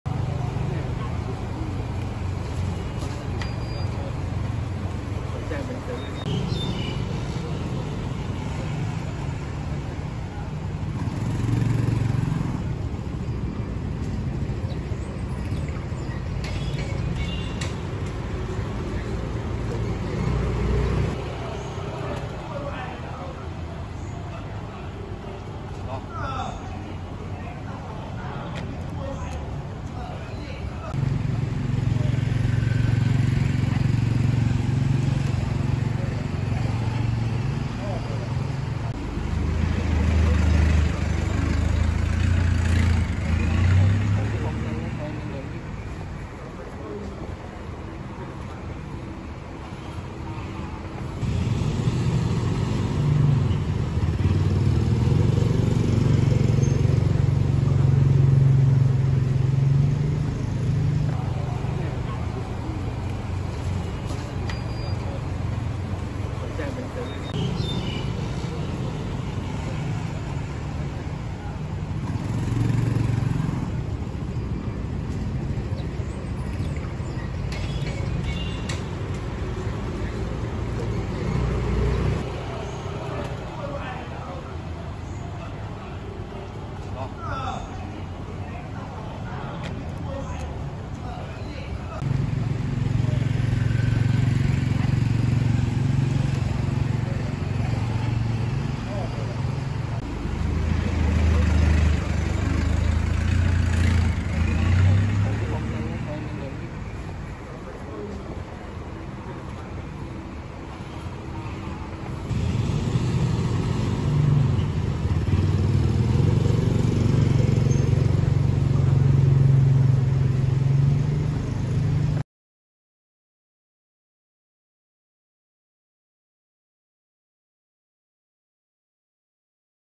ล่าวินจยย.โหดยิงเพื่อนร่วมอาชีพดับ คาดปมแค้นเรื่องส่วนตัว | HOTSHOT เดลินิวส์ 13/04/65
เกิดเหตุยิงกันเสียชีวิต บริเวณปากซอยเสริมสุข ถนนประชาชื่น แขวงวงศ์สว่าง เขตบางซื่อ  กทม. ผู้ตายเป็นคนขับจยย.รับจ้าง โดนเพื่อนร่วมอาชีพขี่จยย.ขวางหน้าก่อนพูดจาหาเรื่อง  จากนั้นก็ใช้ปืนยิงเข้าอกล้มคว่ำ กู้ภัยนำตัวส่งรพ.แต่ทนพิษบาดแผลไม่ไหวเสียชีวิต   ตำรวจเร่งหาเบาะแสวงจรปิดไล่ล่าตัวผู้ก่อเหตุแล้ว